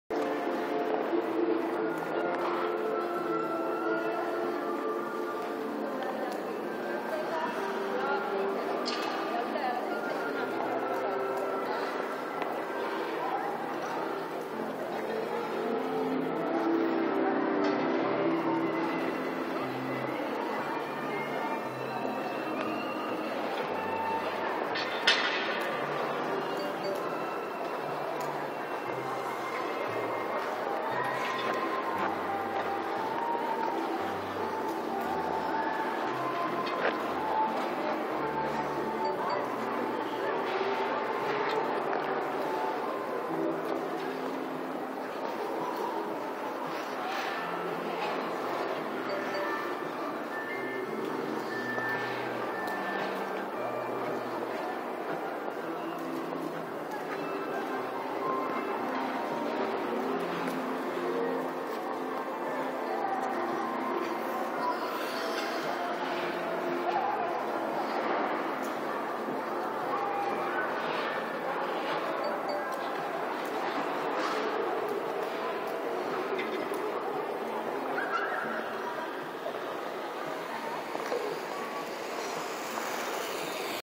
Anni kava 4
Siin kavas on tal elementide asukohad veidi sassis aga muidu kõik olemas.